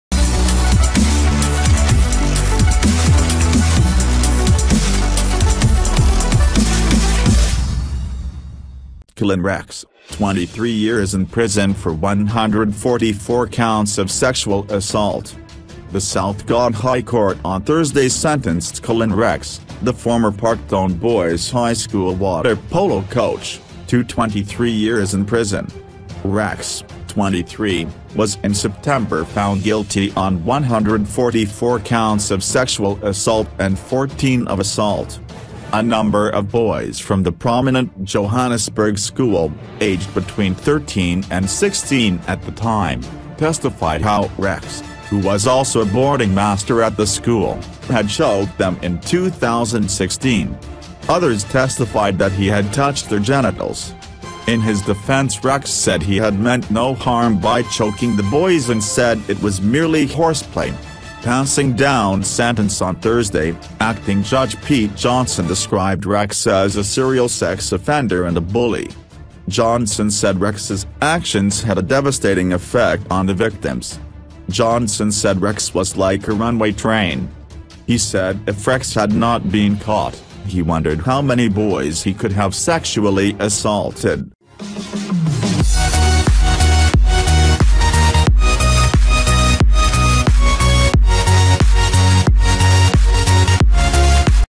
Collan Rex: 23 Years in Prison for 144 Counts of Sexual Assault
Collan Rex: 23 Years in Prison for 144 Counts of Sexual Assault. The South Gauteng High Court on Thursday sentenced Collan Rex, the former Parktown Boys’ High School water polo coach, to 23 years in prison. Rex, 23, was in September found guilty on 144 counts of sexual assault and 14 of assault.
📽 Robert Mugabe at the un 2018